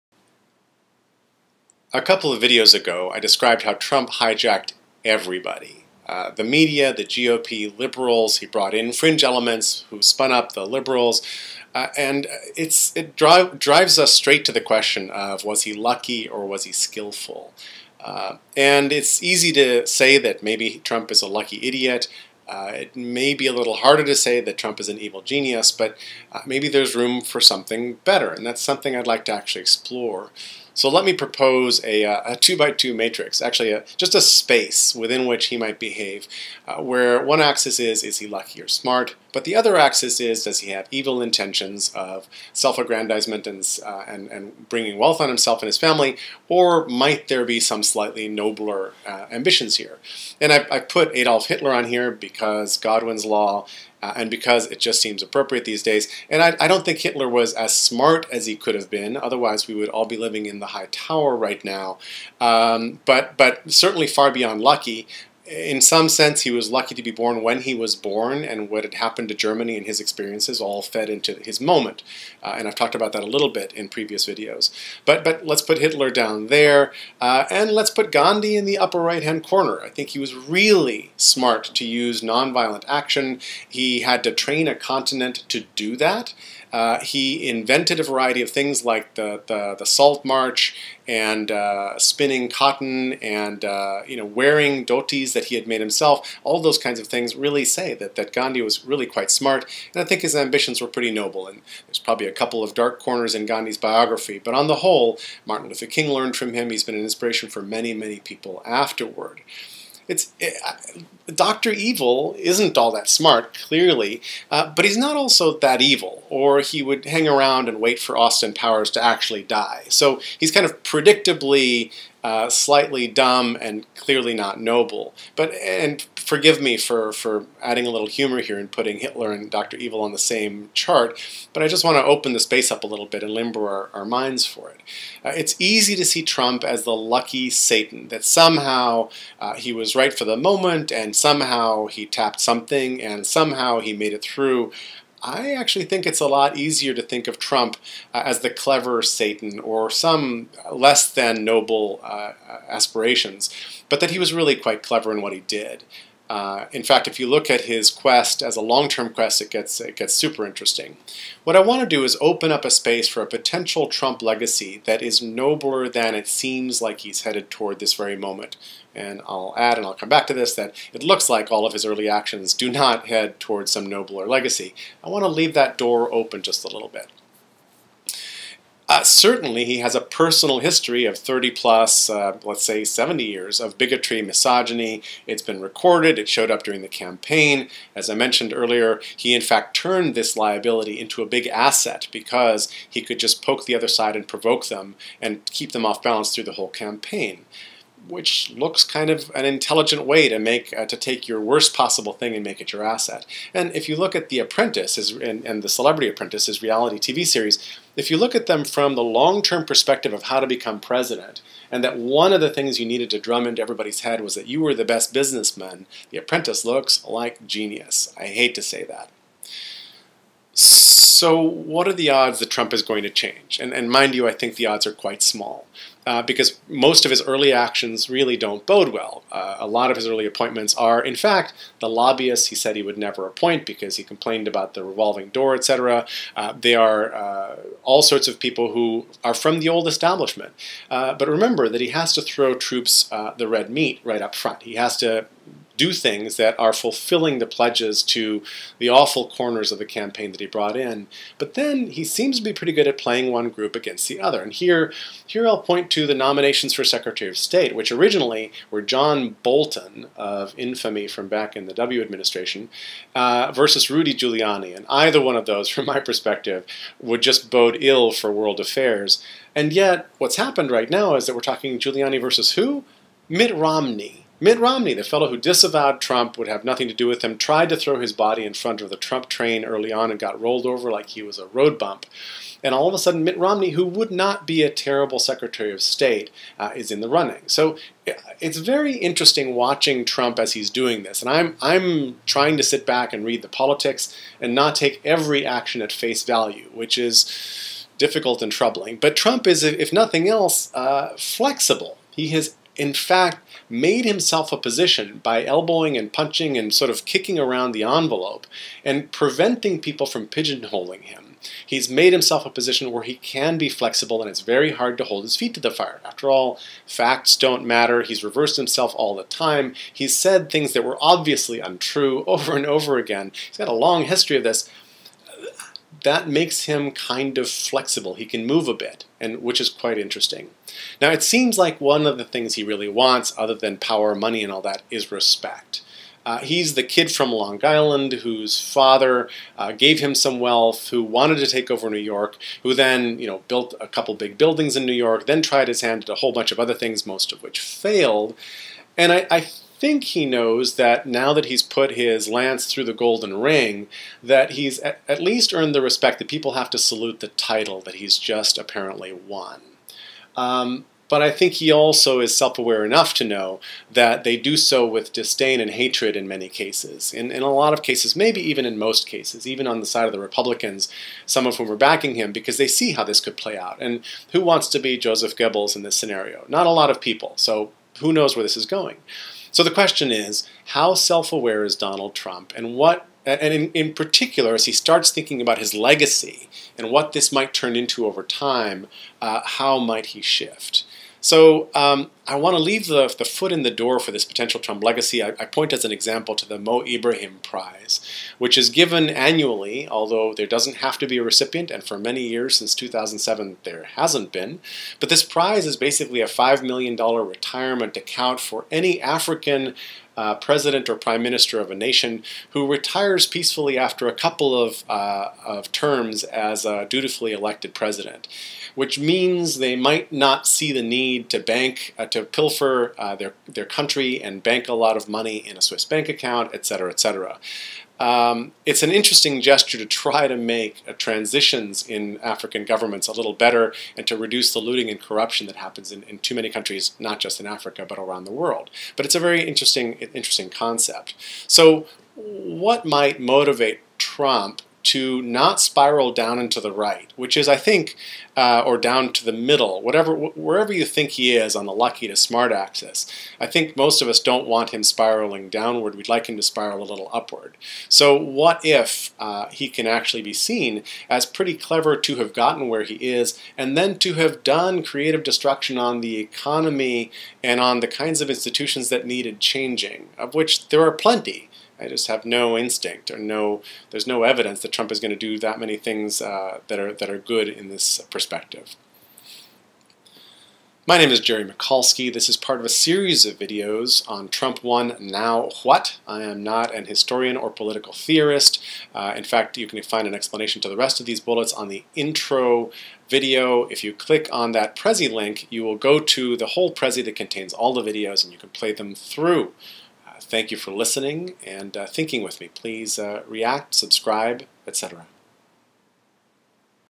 Trump Won (5/6): Evil Genius?
This is one of a series of videos processing Trump's win, which surprised me more than it should have. I'm not only exploring blind spots and tense dynamics, I'm also looking for positive actions we might take. The Prezi I used to create these videos has links, videos (including this one) and more details, and lives